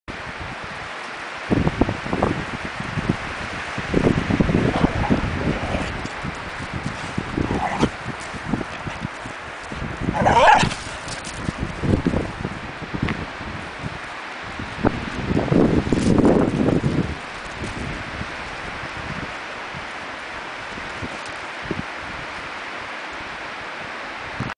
Dogs enjoying a run
Meg, Candy & Poppy enjoying a run along one of the local forestry tracks near Rockchapel, Co. Cork.  Poppy is 7 months old now. She has grown to be bigger than Meg, I doubt if she has much labrador in her at all because she hates getting wet!